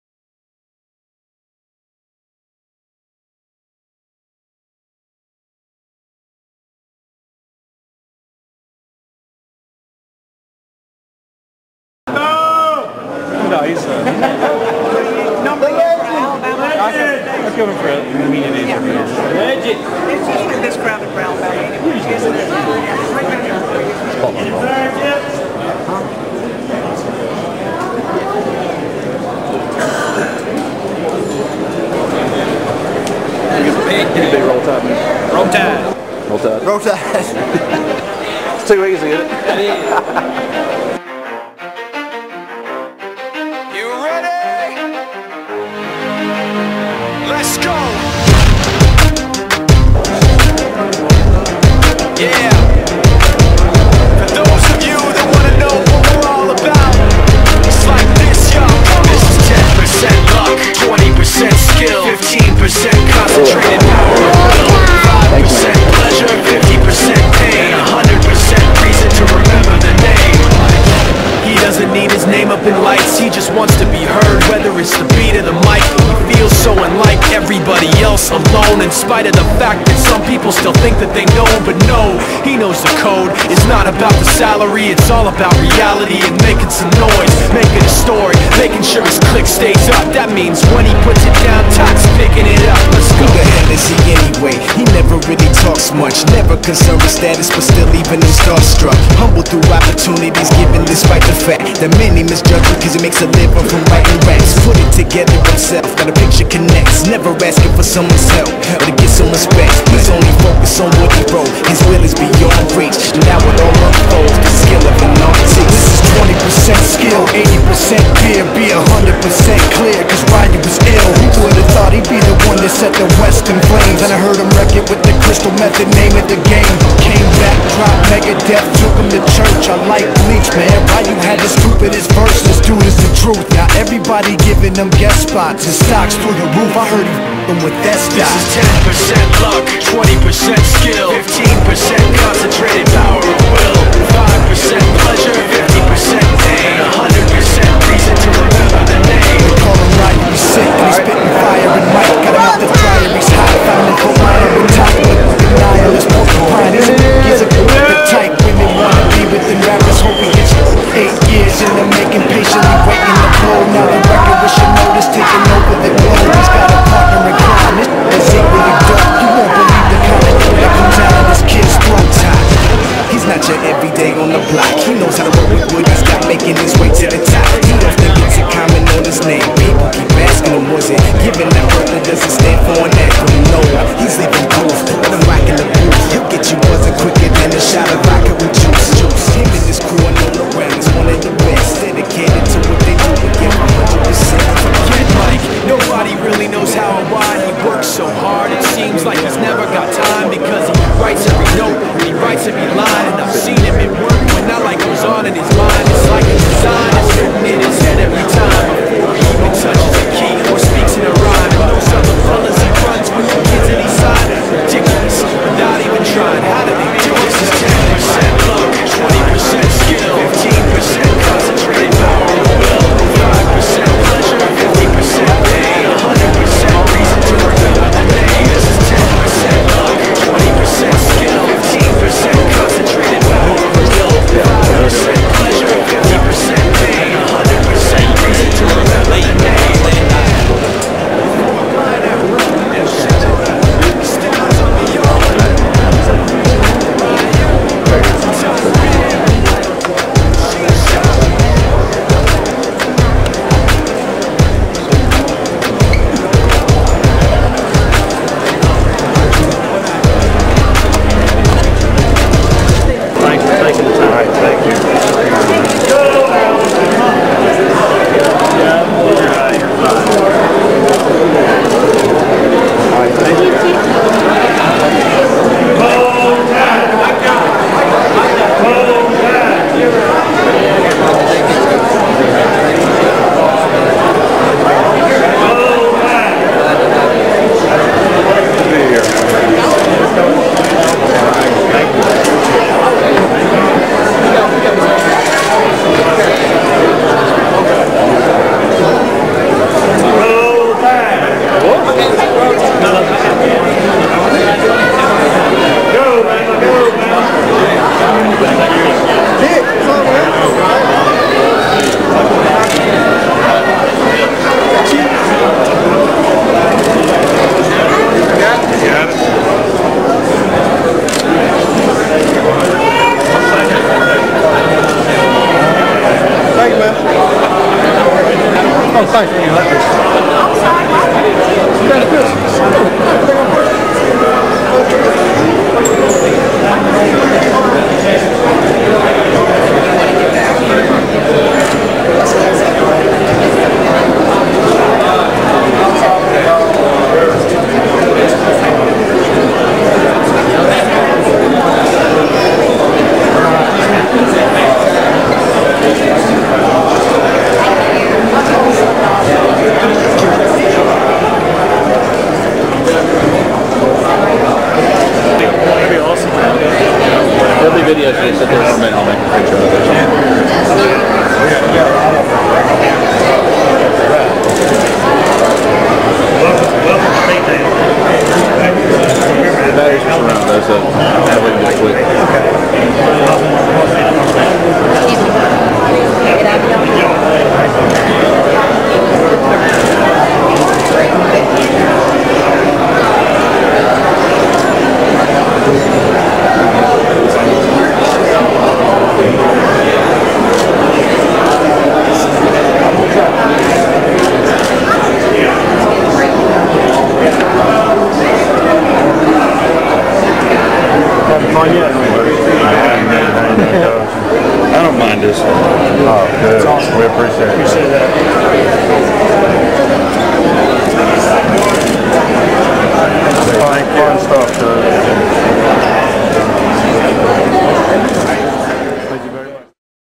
SEC Media Days - University of Alabama Football- 7/26/07
Compilation of shots taken from SEC Media Days during Alabama's time with the media at the Wynfrey Hotel in Birmingham, Alabama.  Nick Saban, Antoine Caldwell, Simeon Castille, Crimson Tide fans. Alabama Football, Roll Tide.